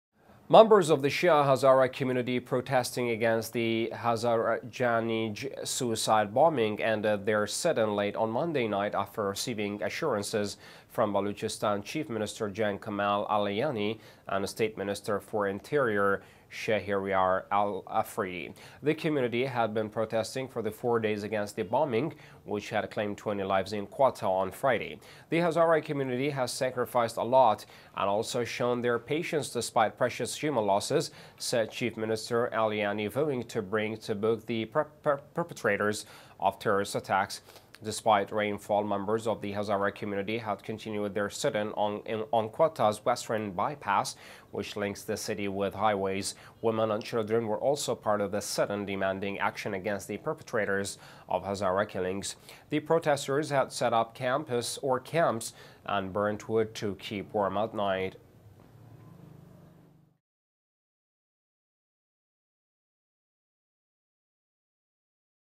Hazara community ends sit-in after receiving assurances from state minister, Balochistan CM
Members of the Shia Hazara community, protesting against the Hazarganji suicide bombing, ended their sit-in late on Monday night after receiving assurances from Balochistan Chief Minister Jam Kamal Alyani and State Minister for Interior, Sheharyar Afridi.
The community had been protesting for the four days against the bombing, which had claimed 20 lives in Quetta on Friday.
"The Haraza community has sacrificed a lot, and also shown their patience despite [the loss of] precious human losses," said Chief Minister Alyani, vowing "to bring to book the perpetrators of terrorist attacks".
Despite rainfall, members of the Hazara community had continued their sit-in on Quetta's Western Bypass, which links the city with highways.
Women and children were also part of the sit-in demanding action against the perpetrators of Hazara killings. The protesters had set up camps and burnt wood to keep warm at night.